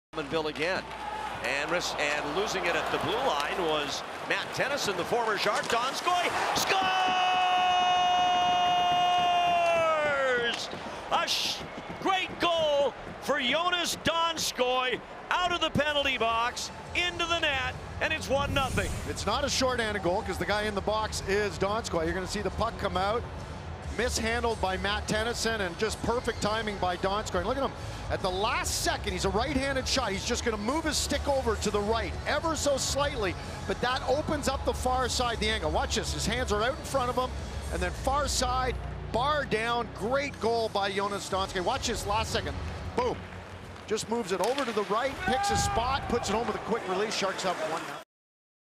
Joonas Donskoi goes bar down on breakaway
Joonas Donskoi jumps out of the box to pick up a loose puck, then races in alone and roofs a wrister past Robin Lehner to put the Sharks up 1-0